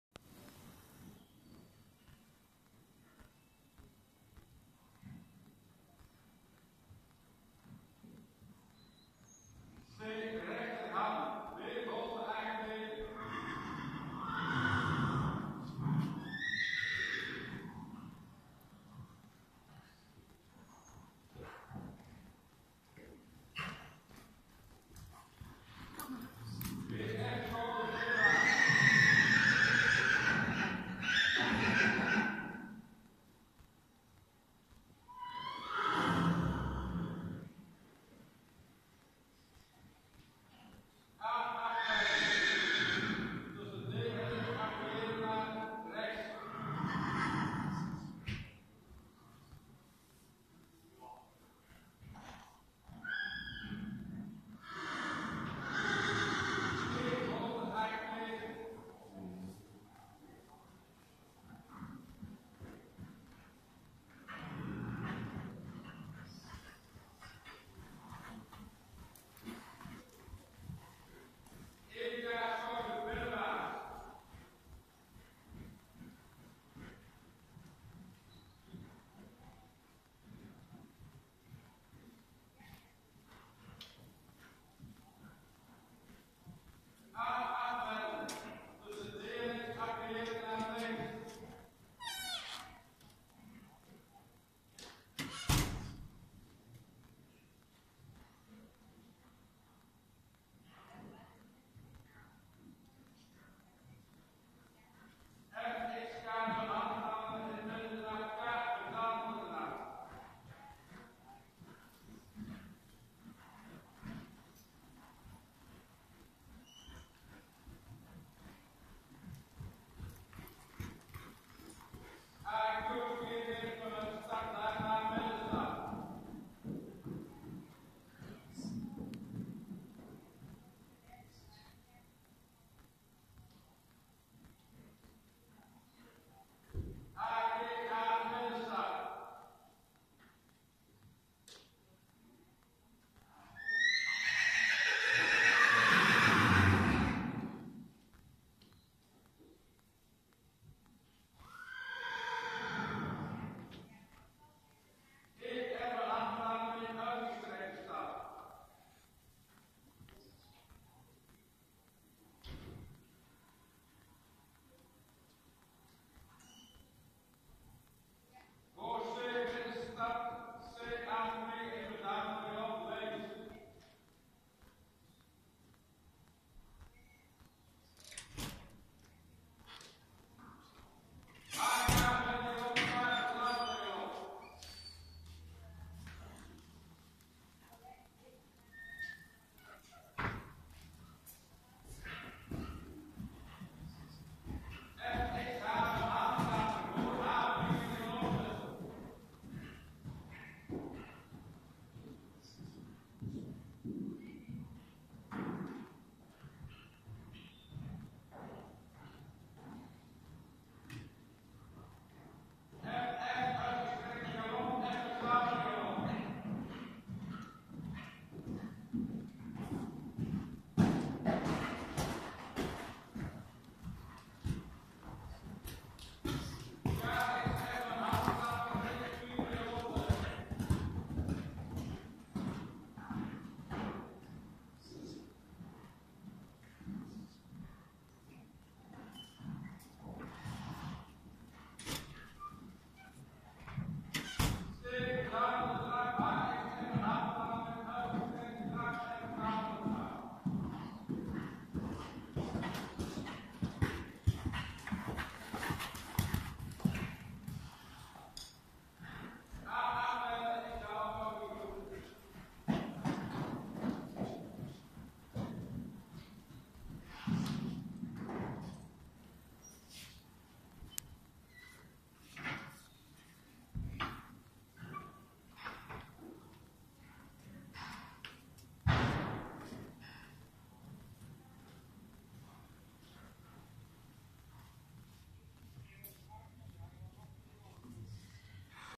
Jelle, Friese hengst (6 jaar): niveau 3.1 te Zutendaal
Dressuurwedstrijden met Jelle, Friese STER hengst: niveau 3.1 te Zutendaal, 3de plaats.